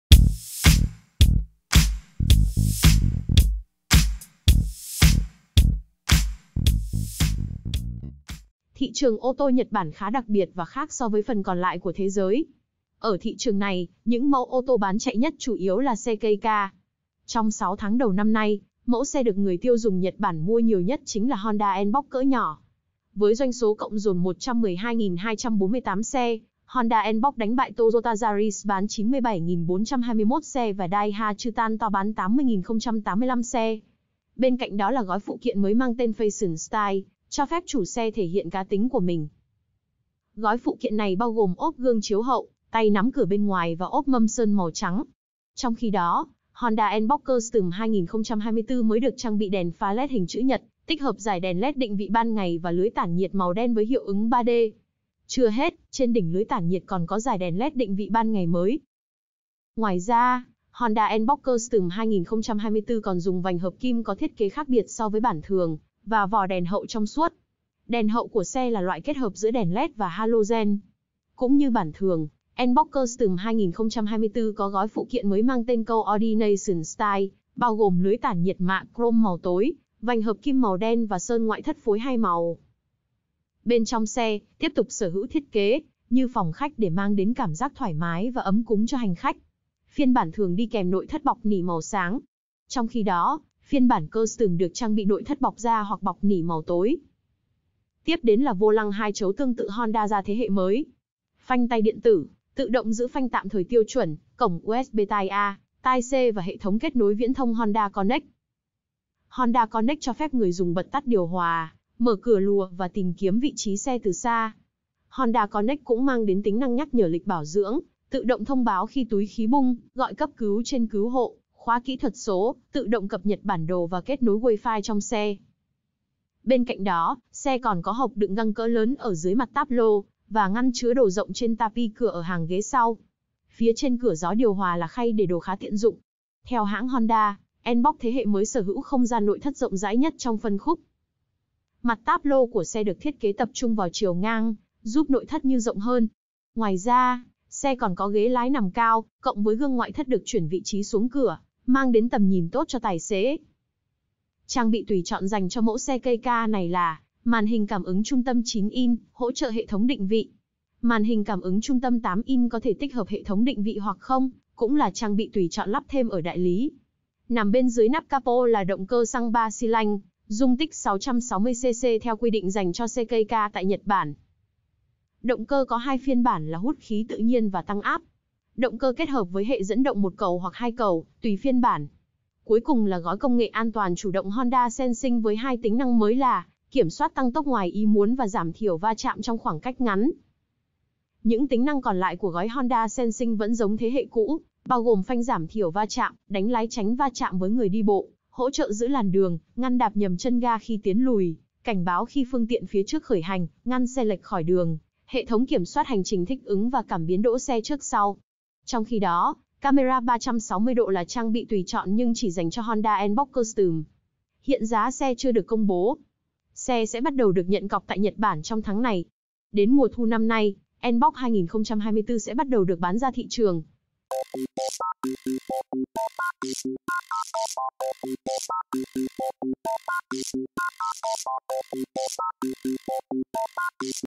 Honda N-Box 2024 kiểu dáng " hộp diêm” nhờ giá rẻ chưa mở bán khách hàng đổ xô đặt mua đến cháy hàng
Mẫu keicar Honda N-Box 2024 được ví như xe hộp diêm” có 2 phiên bản, bao gồm tiêu chuẩn và N-Box Custom, với thiết kế khác nhau nhưng khá ấn tượng.